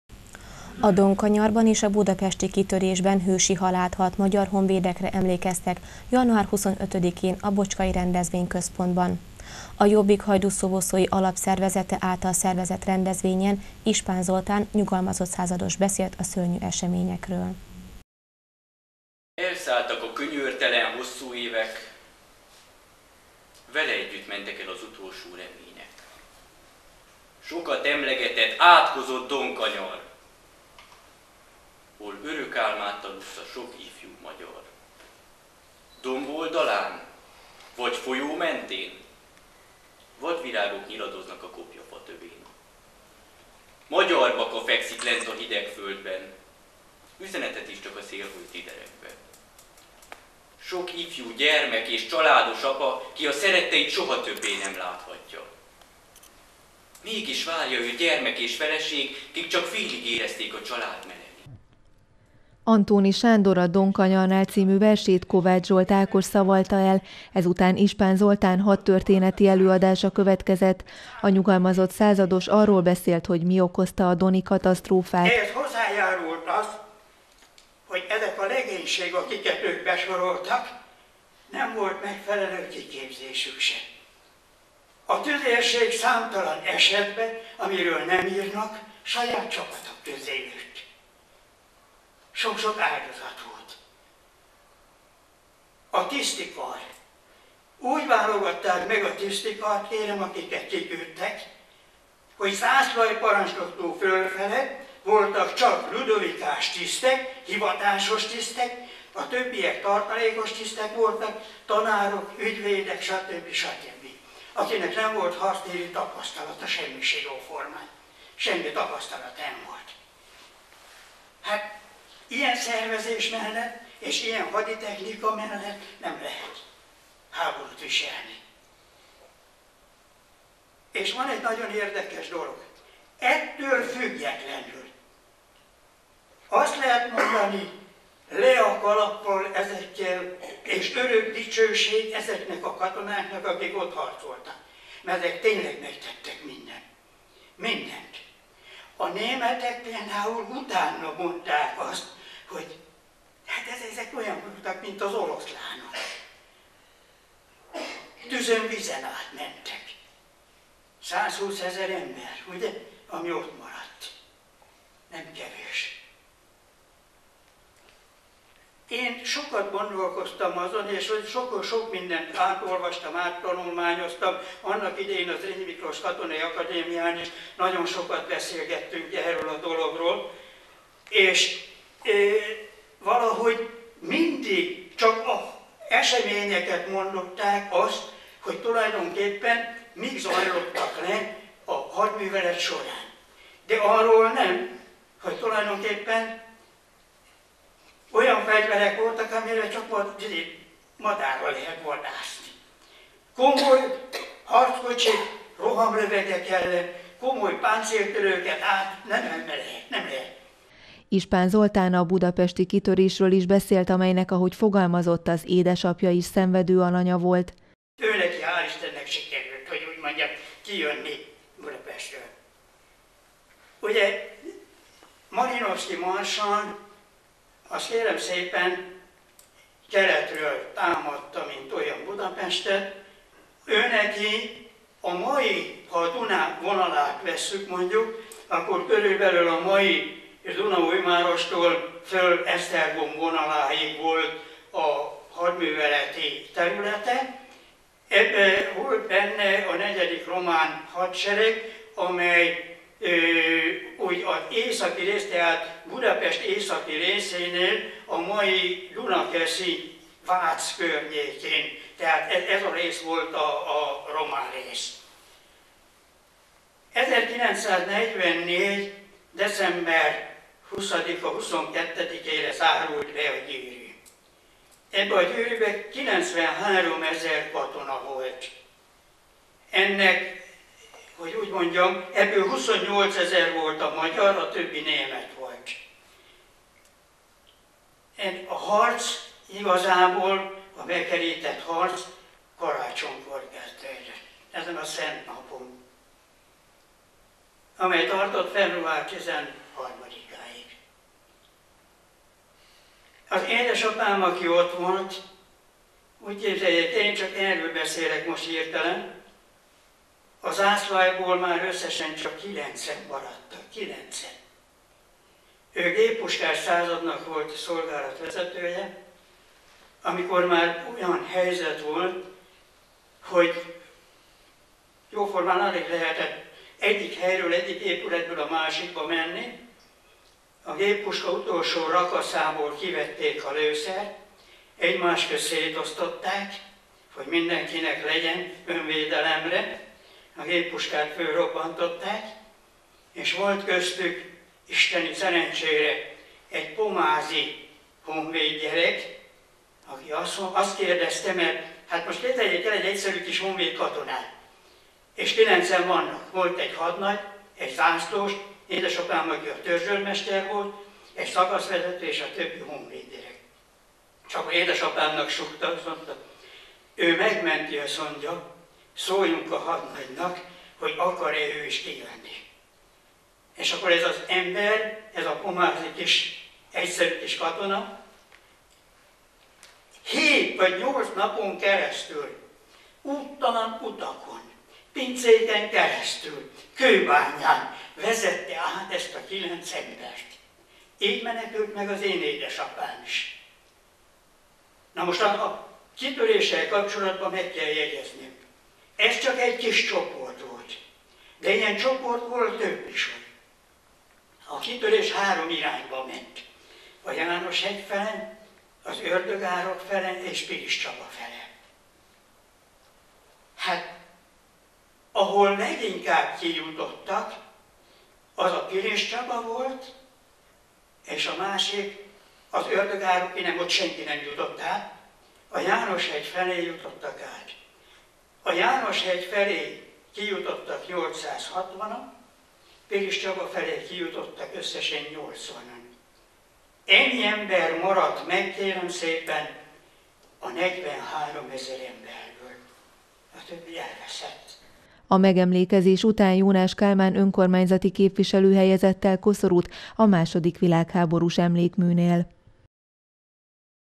A hősökre emlékeztek
A Don-kanyarban és a budapesti kitörésben hősi halált halt magyar honvédekre emlékeztek január 25-én a Bocskai Rendezvényközpontban. A Jobbik Hajdúszoboszlói Alapszervezete által szervezett rendezvényen Ispán Zoltán nyugalmazott százados beszélt a szörnyű eseményekről.